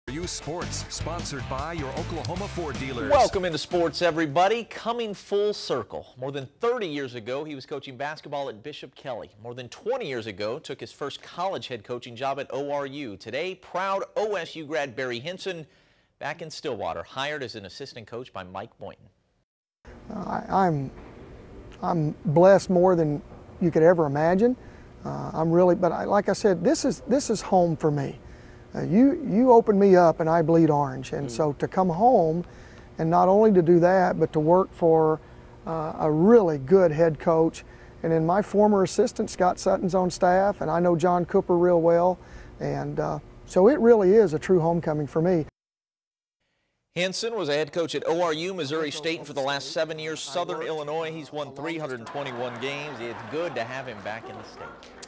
Oklahoma State Basketball adds Barry Hinson to staff
Oklahoma State Basketball adds alum, former ORU Head Coach, Barry Hinson to Mike Boynton's staff